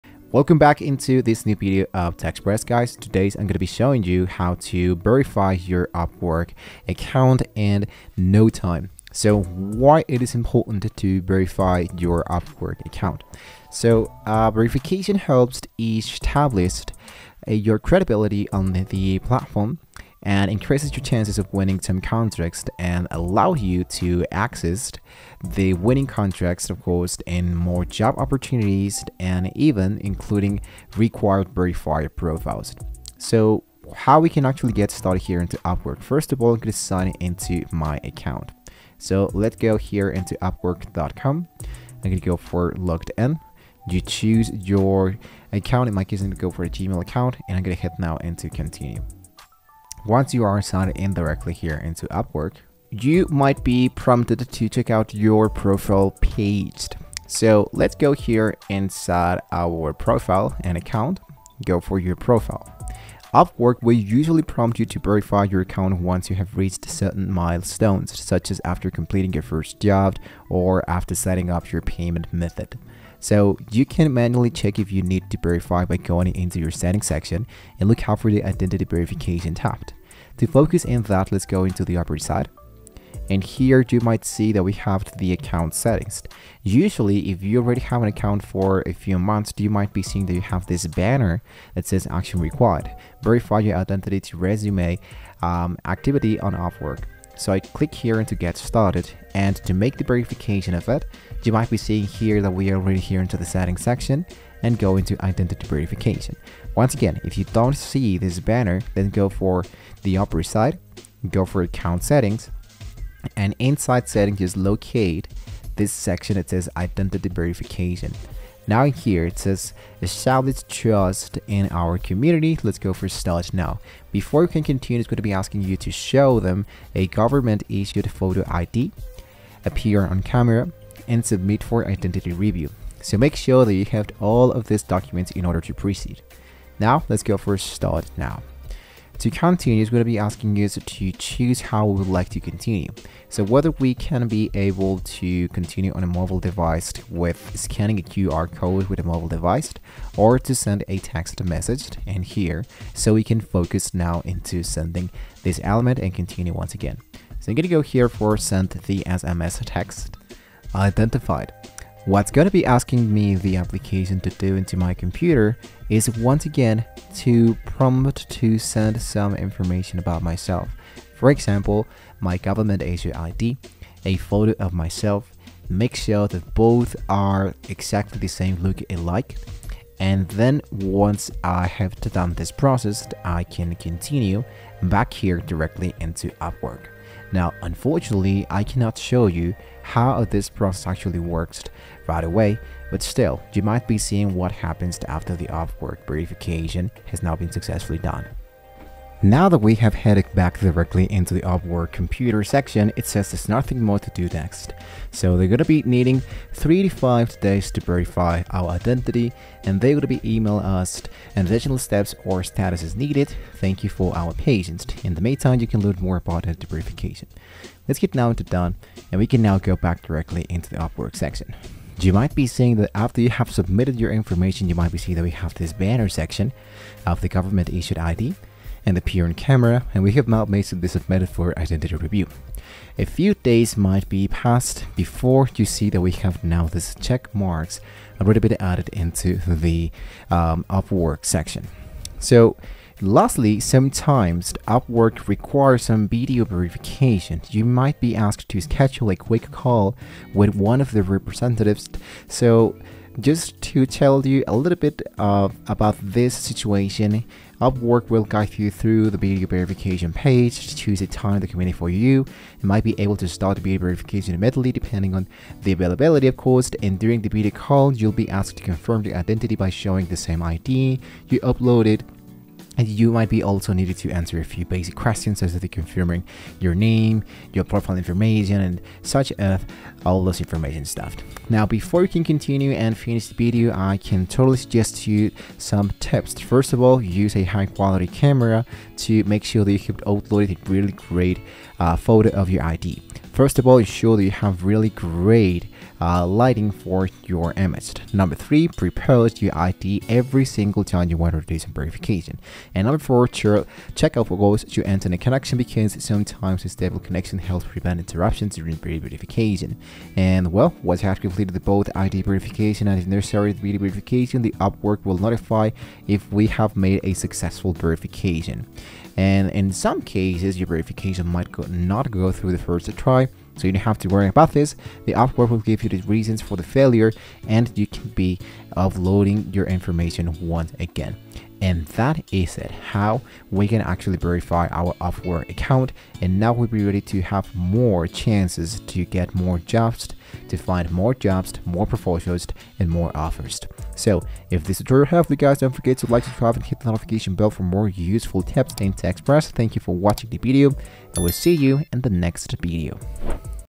How to Verify your Upwork Account (2026) | Tutorial for Beginners Upwork Verification
Learn how to verify your Upwork account quickly and easily with this step-by-step tutorial for beginners. In this video, we’ll guide you through the Upwork verification process, including identity verification and tips to ensure a smooth experience. Whether you’re a freelancer or a client, verifying your account is crucial for building trust on the platform. Follow these simple instructions to complete your Upwork verification and start working with confidence.